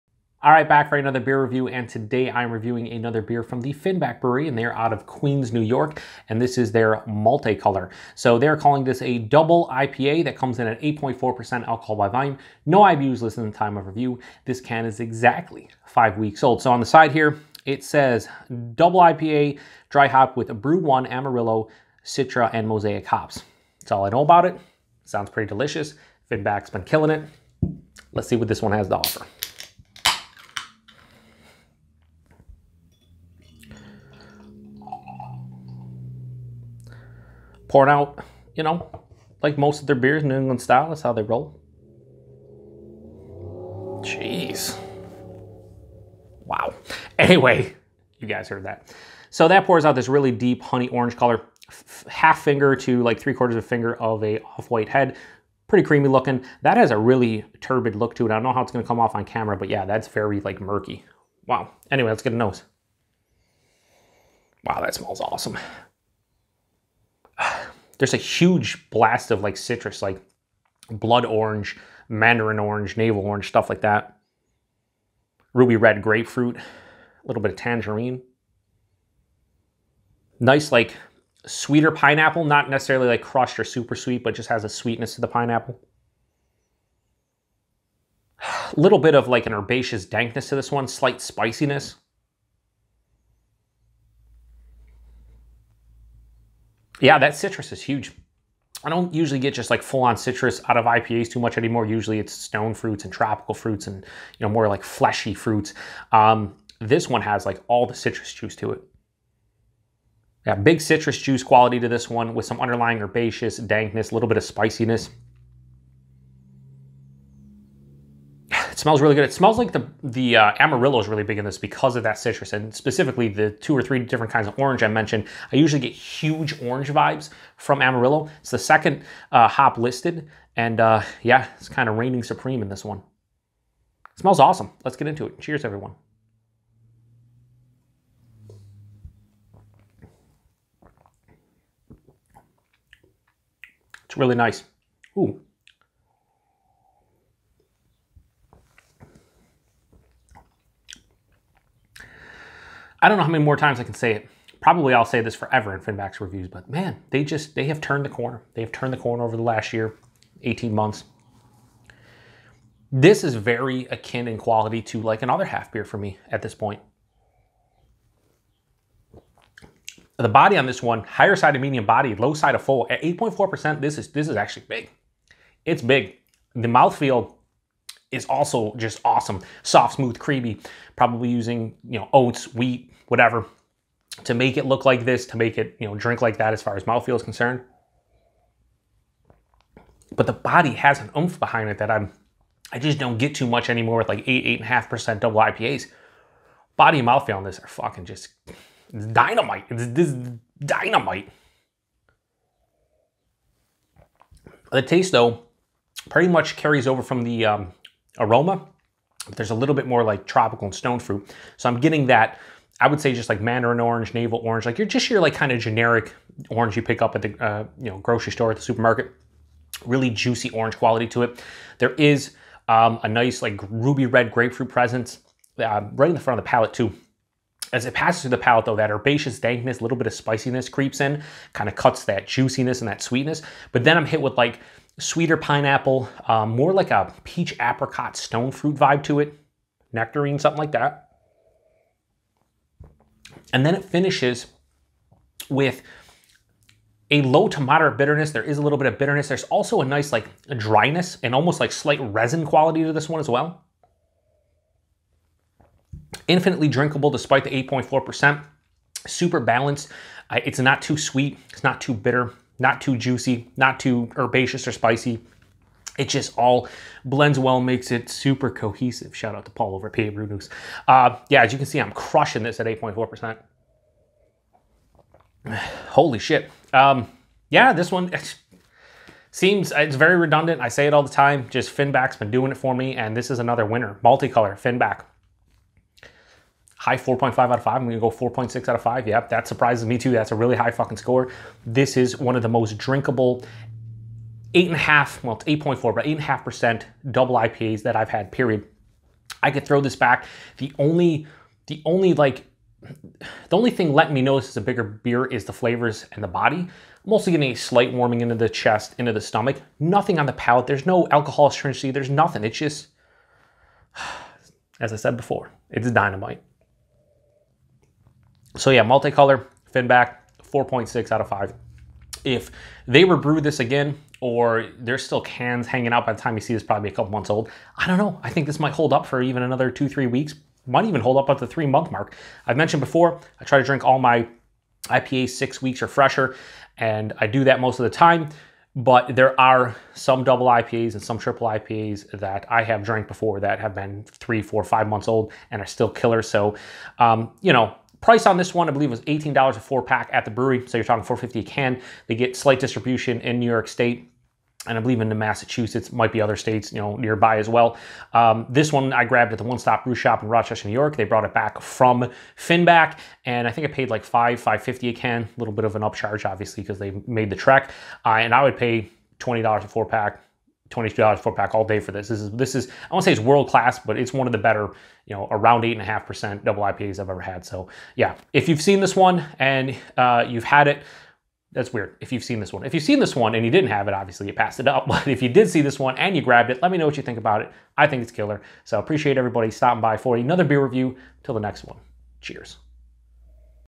Multicolor (DIPA) | Finback Brewery | Beer Review | #456
Today, I'm once again reviewing a beer from the Finback Brewery, and it's their Multicolor.  This is a Double IPA that is dry-hopped with Bru-1, Amarillo, Citra and Mosaic hops.  Does that sound good?  It sure does!  Let's see if it is or not!

Commercial Description:

Multicolor is a 8.4% DIPA dry hopped with Bru-1, Amarillo, Citra and Mosaic.

Pertinent Information:

Brewery - Finback Brewery (Queens, NY)
Beer - Multicolor
Style - Double IPA
ABV - 8.4%
IBU - N/A
Canned On Date - 05/02/19 (Exactly five weeks old at the time of review)